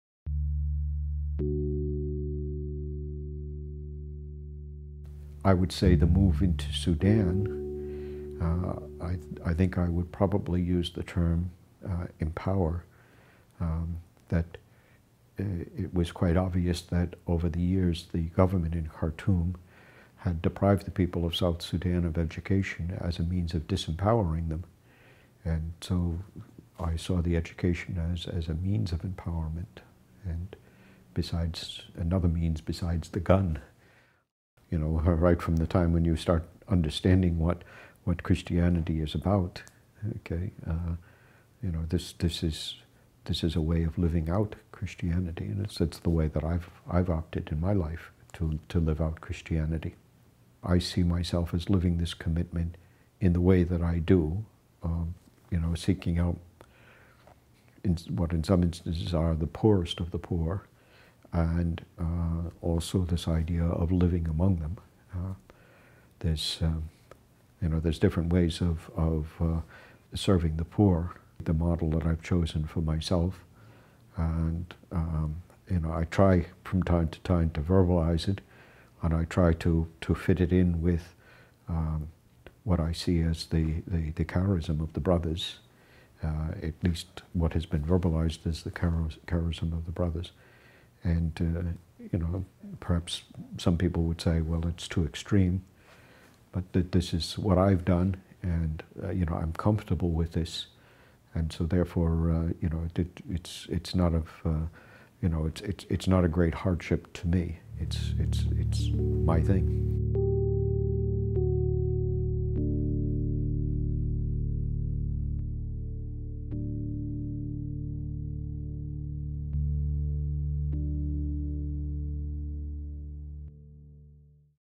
Brother Mike Foley, C.F.X. on Living Among the Poor
Brother Mike Foley lives and works in Narus, South Sudan. In this video he talks about his commitment to the people he works with in South Sudan.